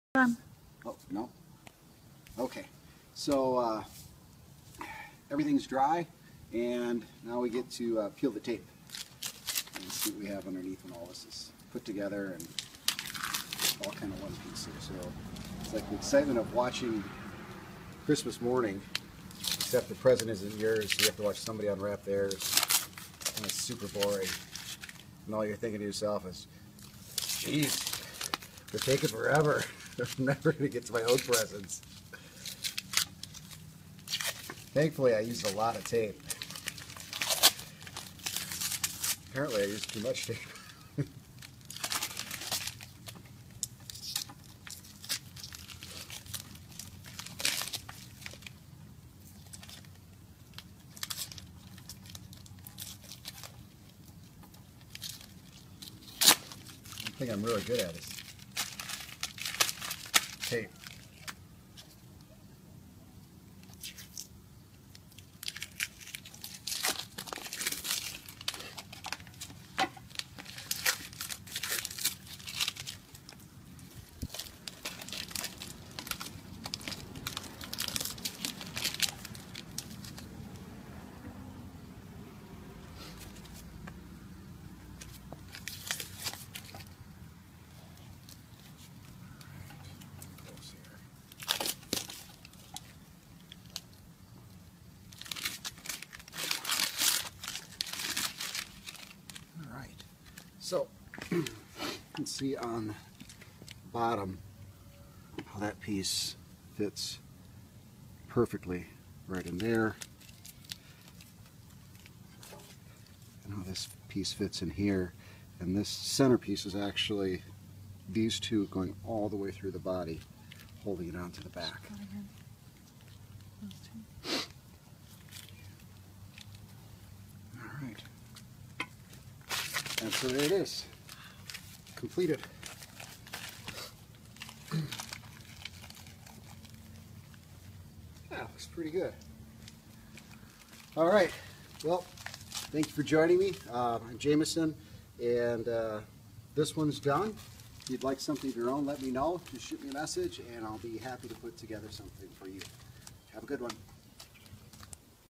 Tortoise Sculpture - Step 8
Unveiling of custom tortoise art sculpture.

Custom ceramic and steel tortoise art sculpture designed and created by Jamison Glisczinski of Jamison Studios, an art studio in Neenah, Wisconsin. This custom art piece will be utilized as outdoor garden art.